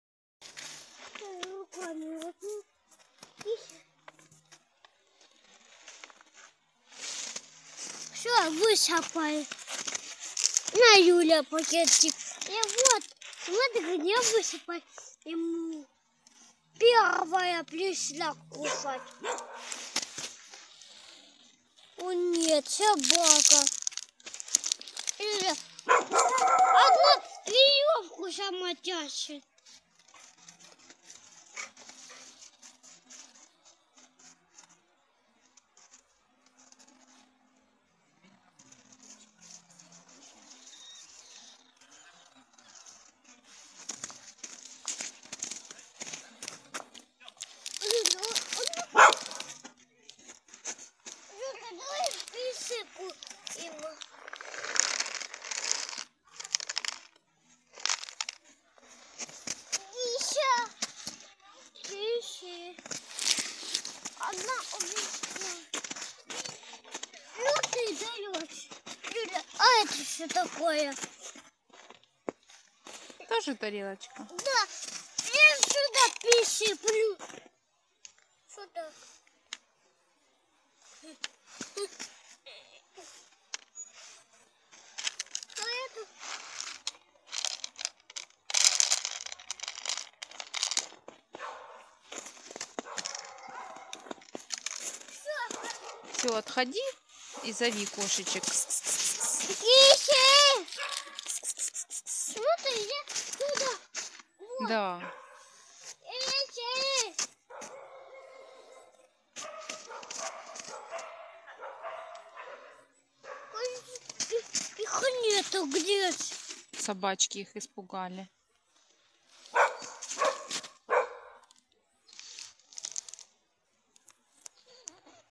Бездомные домашние животные зимой.
Расскажем  и покажем как дети кормят бездомных кошек и собак. Пытаясь угодить кошечкам, ребенок пересыпает корм из миски в миску.